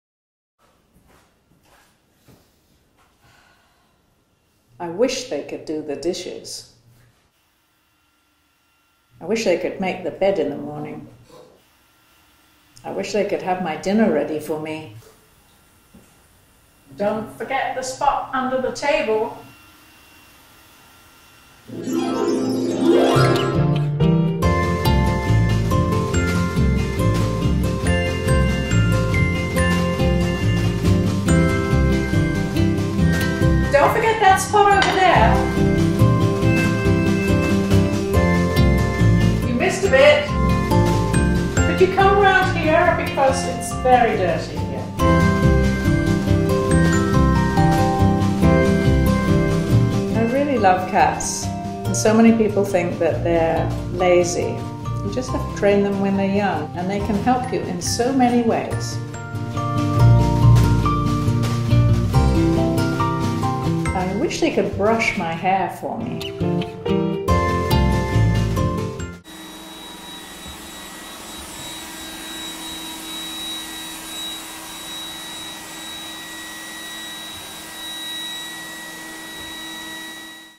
How Welsh Model Grace Coddington Puts Cats to Work Around the House | Architectural Digest
How Welsh model Grace Coddington puts cats to work around the house.

Transcript
 I wish they could do the dishes.
 I wish they could make the bed in the morning.
 I wish they could have my dinner ready for me.
 Don't forget the spot under the table.
 Don't forget that spot over there.
 You missed a bit.
 Could you come around here because it's very dirty here.
 I really love cats.
 So many people think that they're lazy.
 You just have to train them when they're young,
 and they can help you in so many ways.
 I wish they could brush my hair for me.